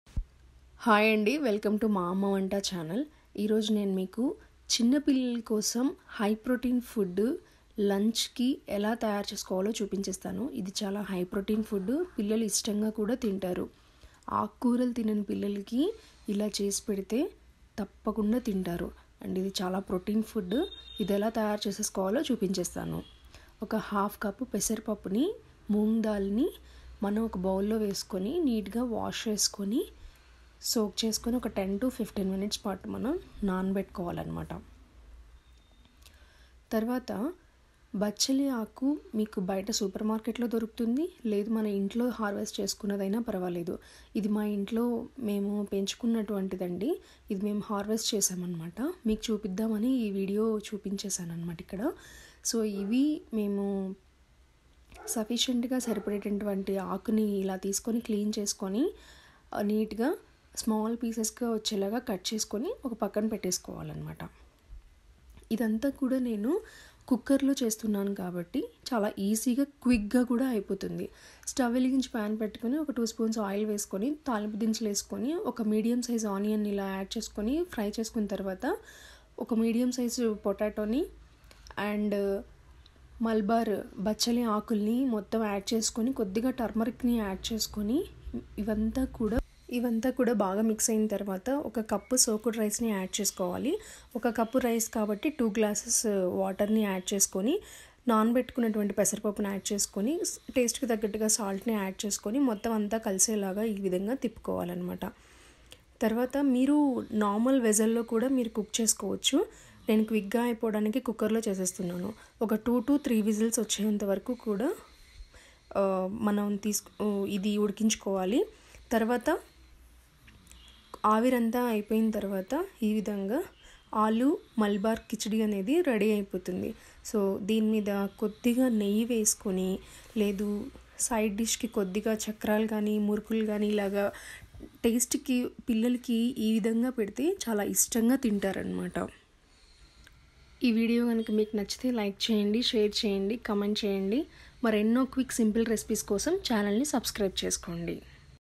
Aloo Malbar kichidi | Potato with బచ్చలి ఆకుకూర తో। పిల్లలికి ఆకుకూర తో ఇలా చేసేయండి।malbar
How to make aloo malbar kchidi